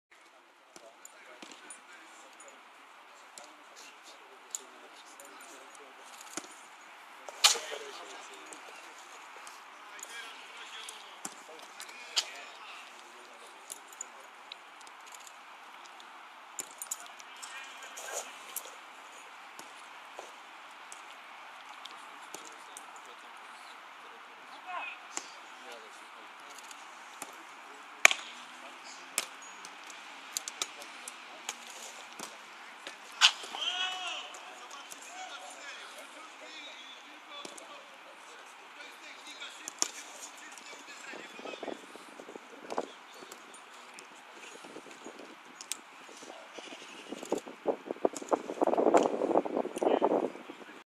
Trening strzelecki Legii Warszawa
Na piątkowych zajęciach (11.10.13) legioniści oddawali strzały. Ładnymi uderzeniami popisywali się Tomasz Brzyski i Helio Pinto.